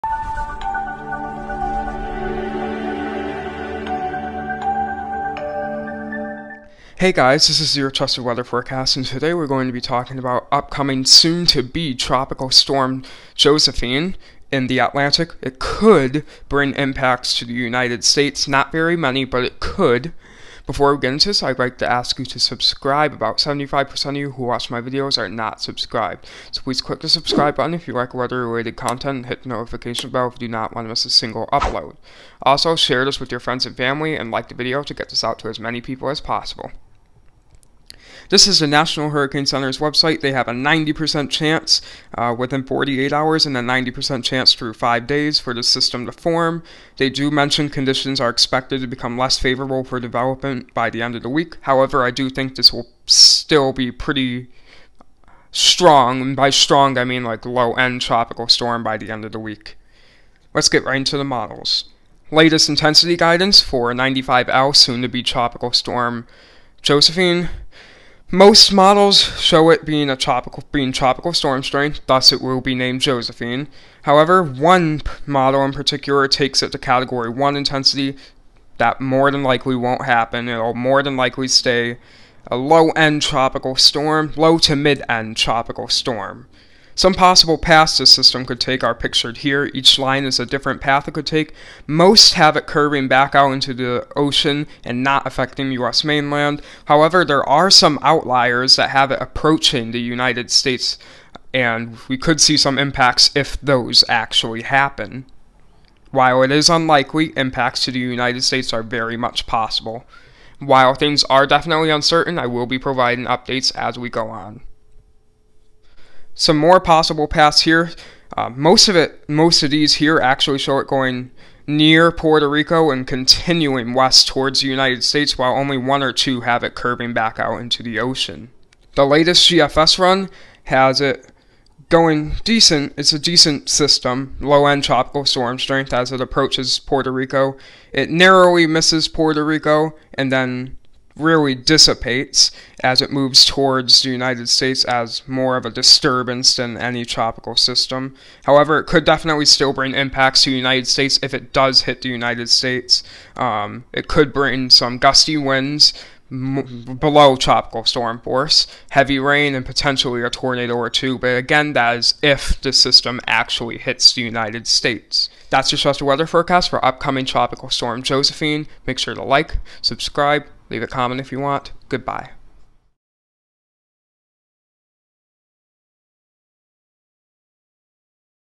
Tropical Storm Josephine Forecast
Invest 95L going to become tropical Storm Josephine later today or tonight as it slowly moves towards the Caribbean islands. Josephine is not expected to make any major impacts on land, as it is likely that it will curve back out into the ocean before any land interaction. Rip currents and high tides will be the main threats with this system, unless it moves over land.